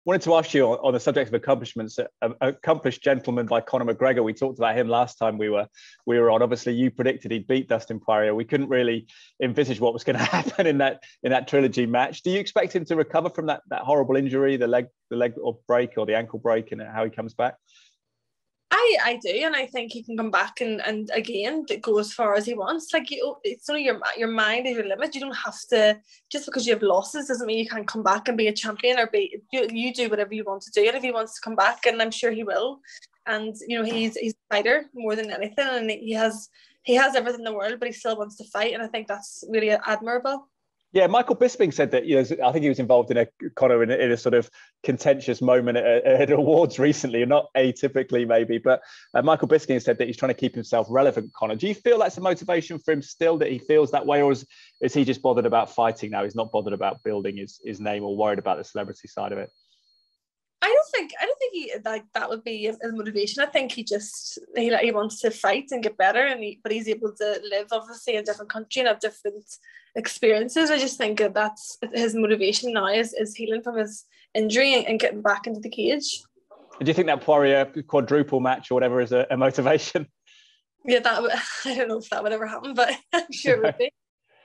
“He still wants to fight!” | Will we see Conor McGregor back in the UFC Octagon? | McCourt discusses
Bellator’s Leah McCourt tells Ed Draper that Conor McGregor still has the motivation to fight in the UFC and believes that he’ll be back and go as far as he wants…